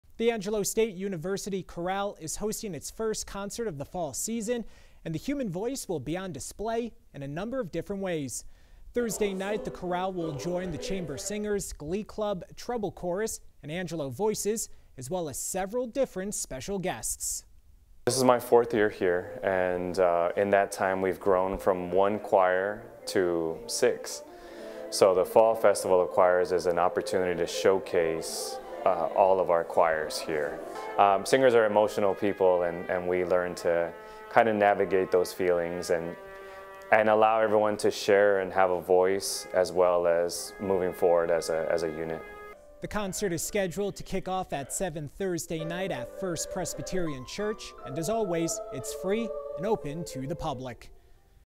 A "Festival of Choirs"
Thursday's concert will feature six Angelo State choirs performing a variety of musical styles.